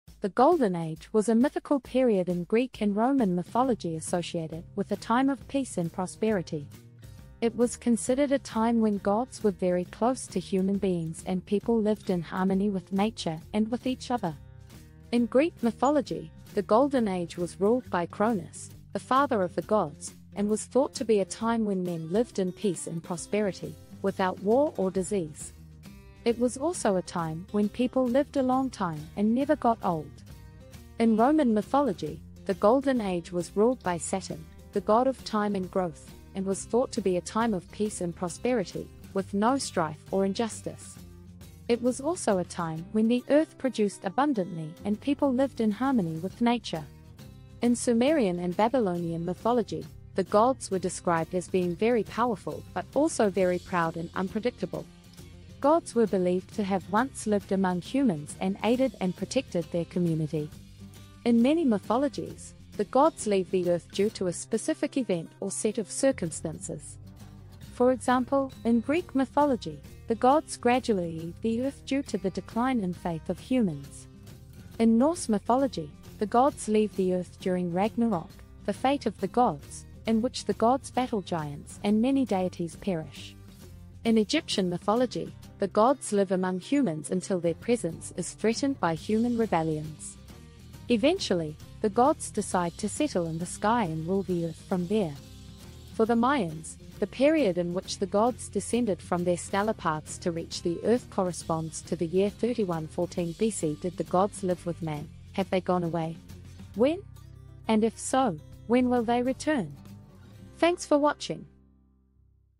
Golden Age When Man Never Grew Old - Big Fire
The Golden Age was a mythical period in Greek and Roman mythology associated with a time of peace and prosperity. It was considered a time when gods were very close to human beings and people lived in harmony with nature and with each other.

In Greek mythology, the Golden Age was ruled by Cronus, the father of the gods, and was thought to be a time when men lived in peace and prosperity, without war or disease. It was also a time when people lived a long time and never got old.

In Roman mythology, the Golden Age was ruled by Saturn, the god of time and growth, and was thought to be a time of peace and prosperity, with no strife or injustice. It was also a time when the earth produced abundantly and people lived in harmony with nature.
In Sumerian and Babylonian mythology, the gods were described as being very powerful but also very proud and unpredictable. Gods were believed to have once lived among humans and aided and protected their community.
In many mythologies, the gods leave the earth due to a specific event or set of circumstances. For example, in Greek mythology, the gods gradually leave the earth due to the decline in faith of humans.

In Norse mythology, the gods leave the earth during Ragnarok, the "fate of the gods", in which the gods battle giants and many deities perish.

In Egyptian mythology, the gods live among humans until their presence is threatened by human rebellions. Eventually, the gods decide to settle in the sky and rule the earth from there.
For the Mayans, the period in which the gods descended from their stellar paths to reach the earth corresponds to the year 3114 B.C. Did the gods live with man, have they gone away? when ? And if so, when will they return?